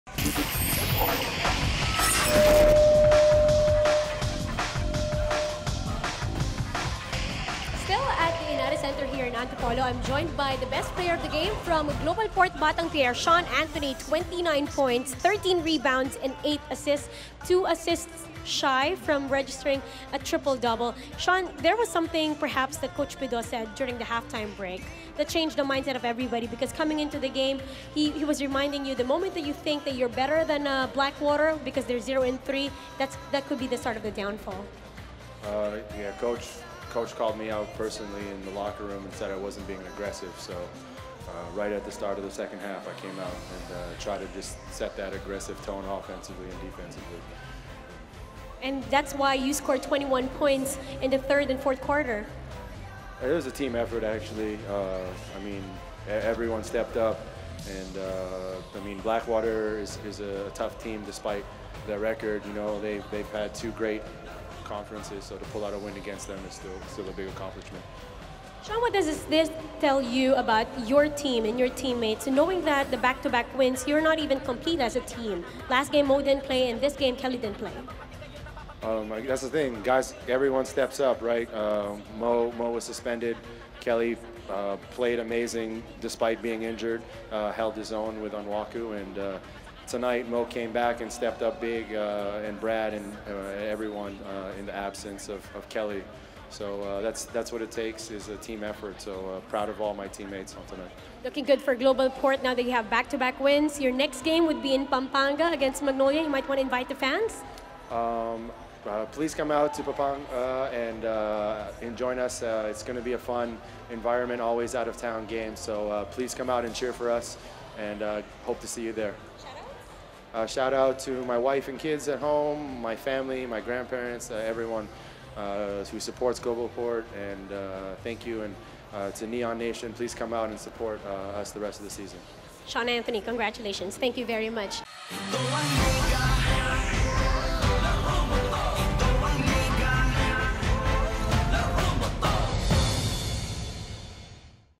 Best Player: Sean Anthony | PBA Commissioner’s Cup 2018
Sean Anthony was named Best Player of GlobalPort vs. Blackwater game.